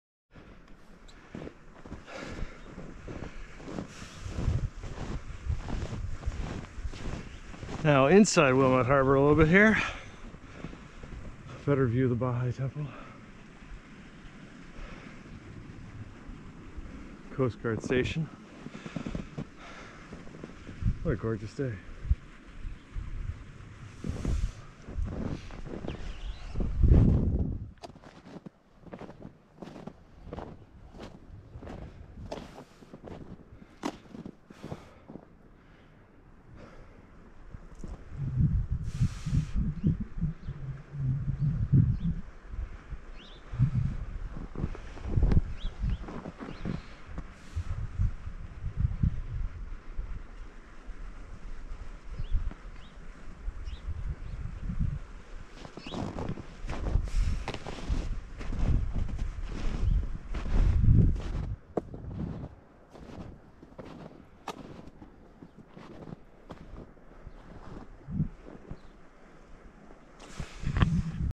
Lake Michigan, IL - Wilmette, IL Harbor and Baha'i Temple in winter (Hike 360° VR Video)
A great view of the Wilmette Harbor and Baha'i Temple in winter. Use the 360° video controls to spin around and see Gillson Park. For more information about the Baha'i Temple,